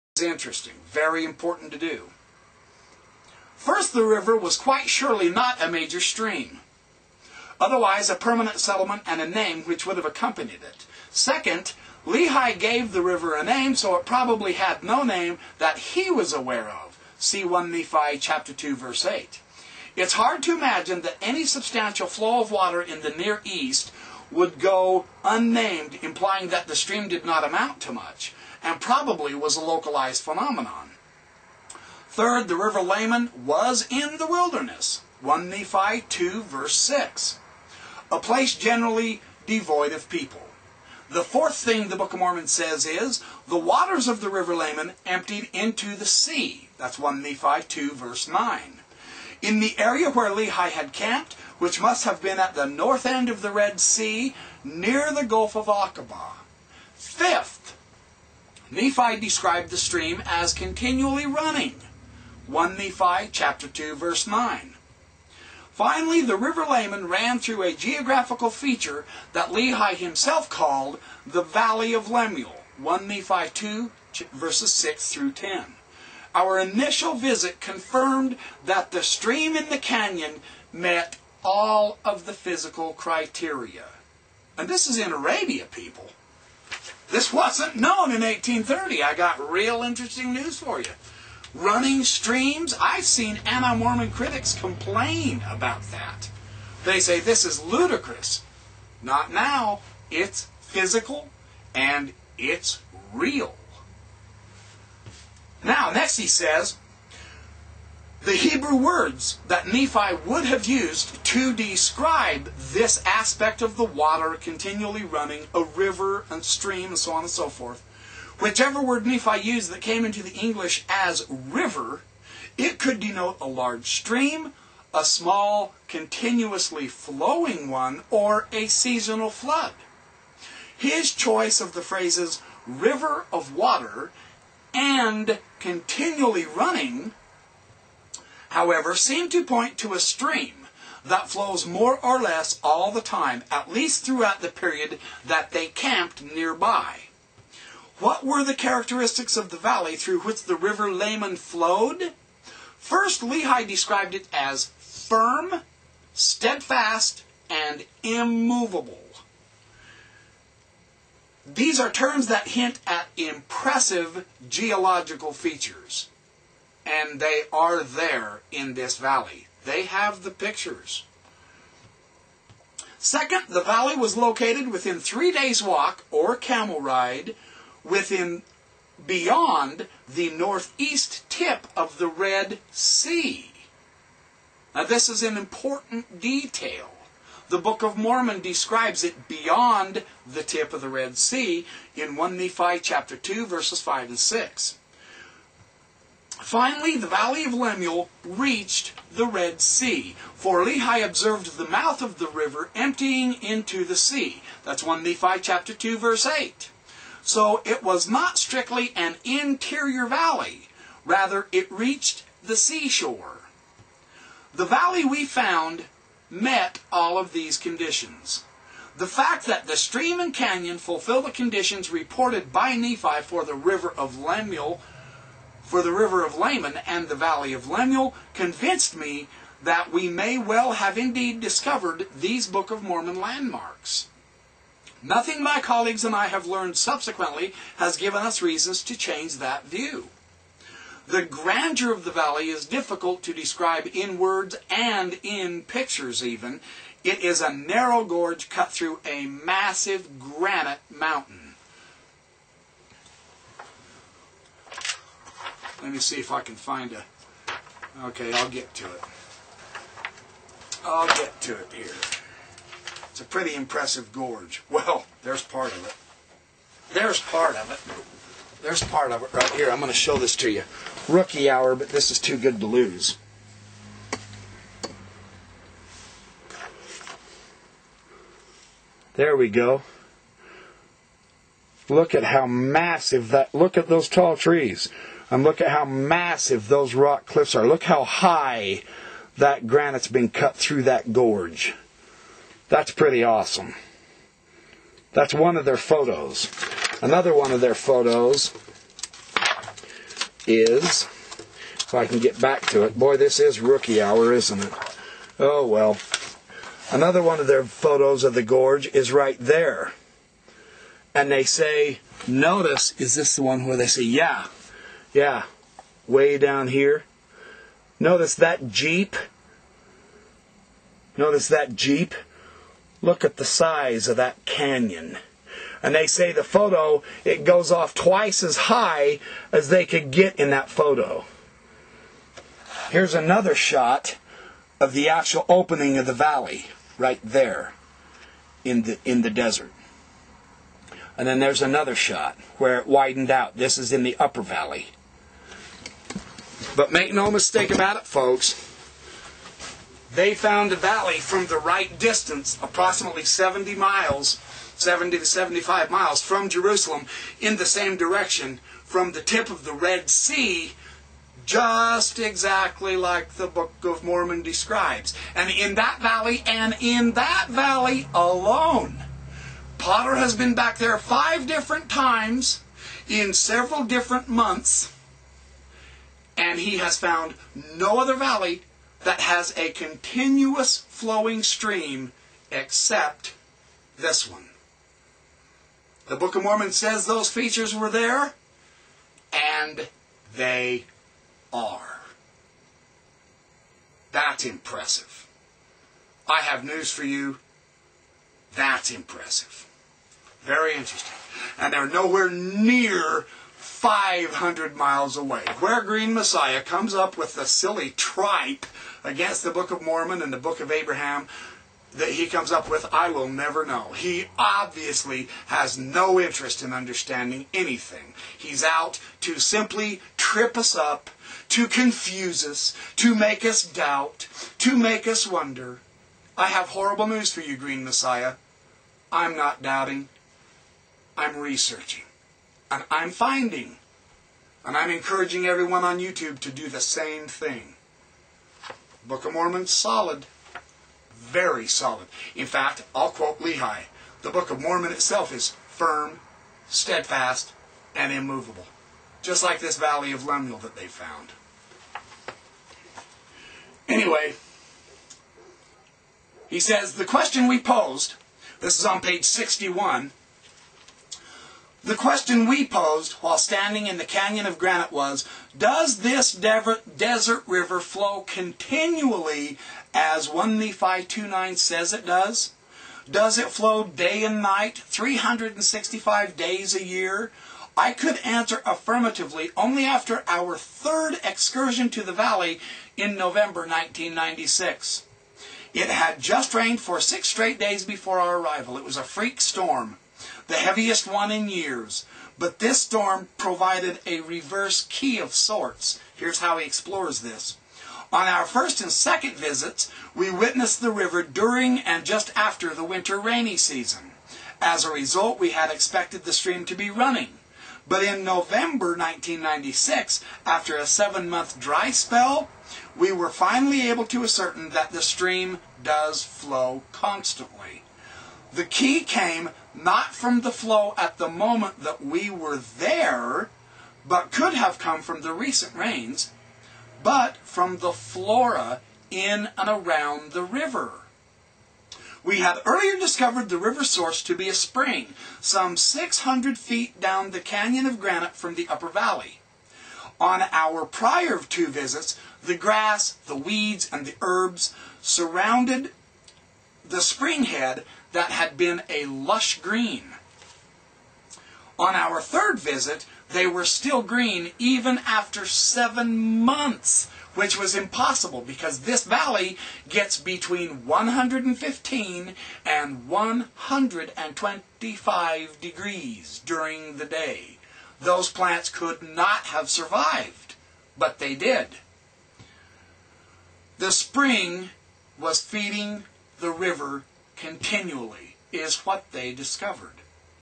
Pt 2 Valley of Lemuel in the Book of Mormon Discovered?
Amazing information on the Valley of Lemuel as well as the River of Laman in real geographical terms in Arabia.